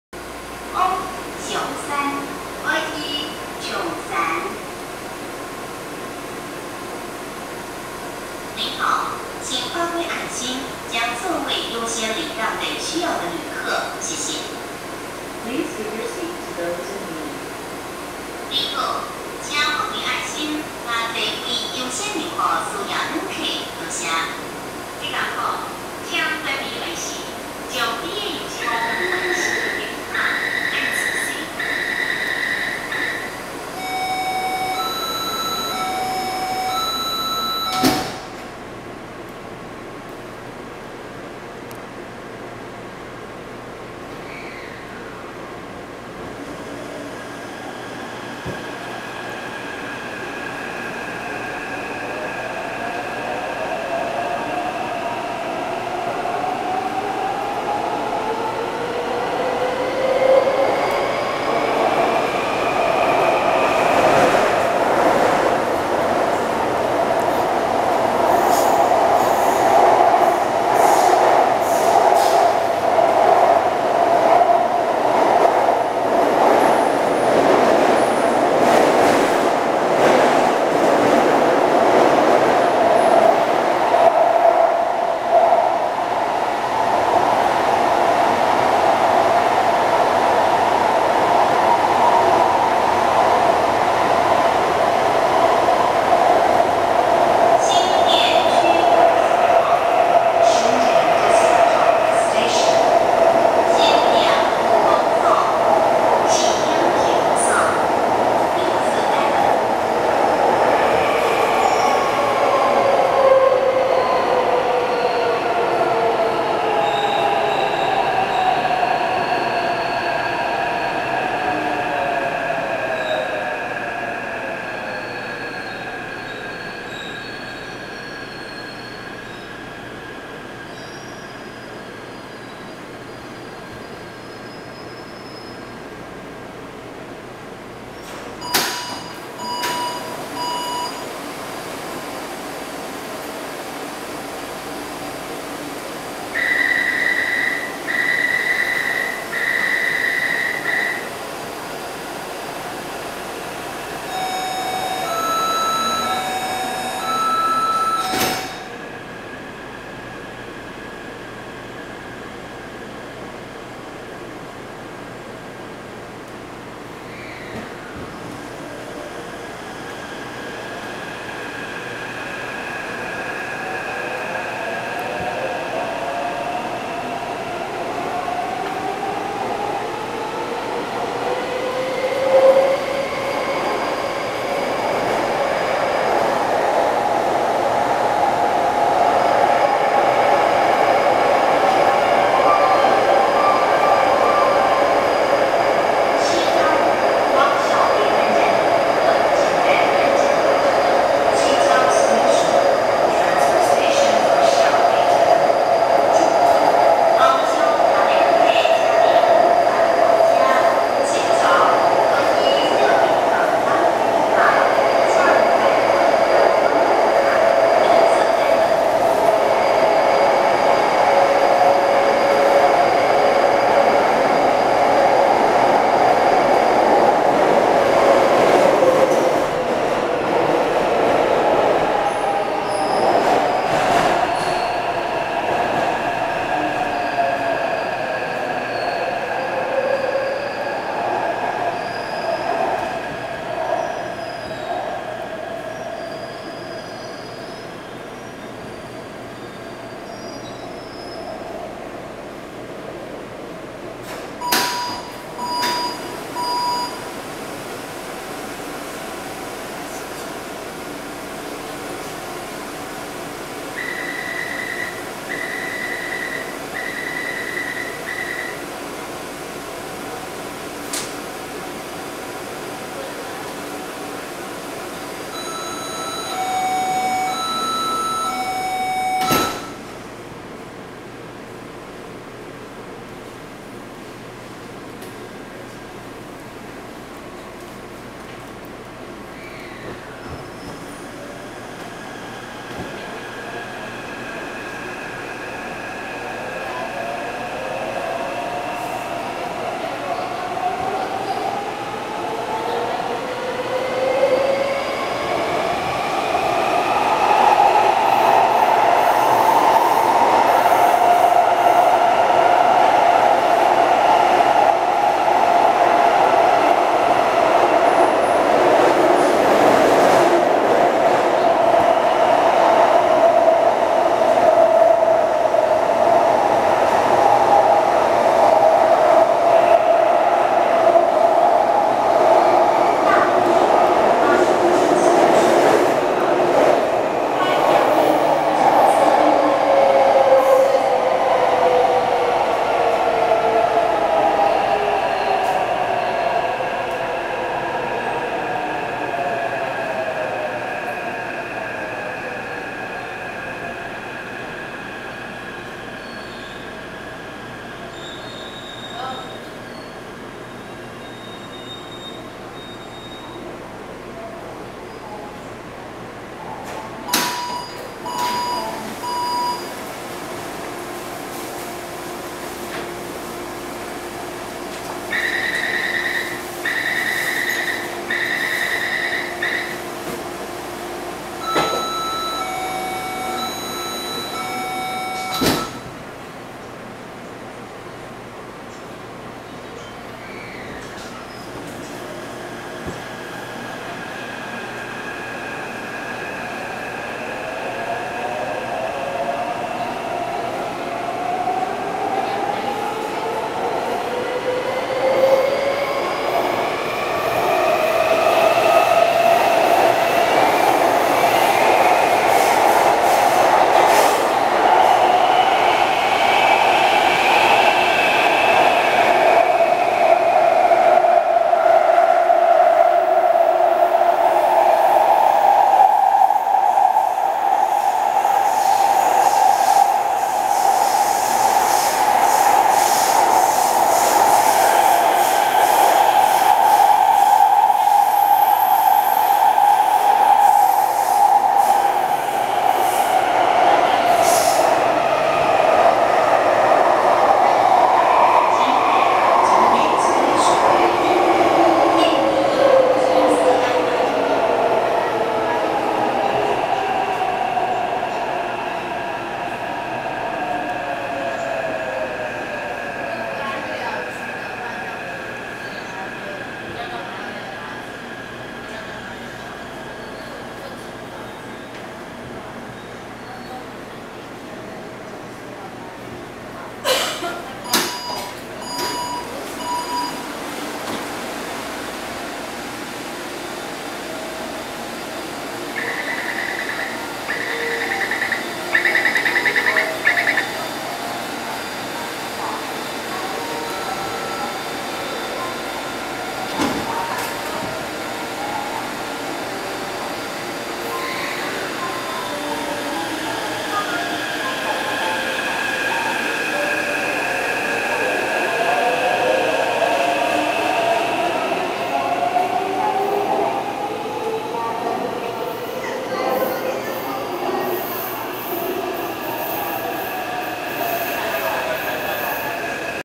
|交通迷天地| [台北捷運] C371-3系更新車 337/338 往G19松山 G01新店~G05景美+離站
拍攝日期:2019年8月5日
拍攝路線:G松山新店線
車次: 317
車號: 337/338
車況:待發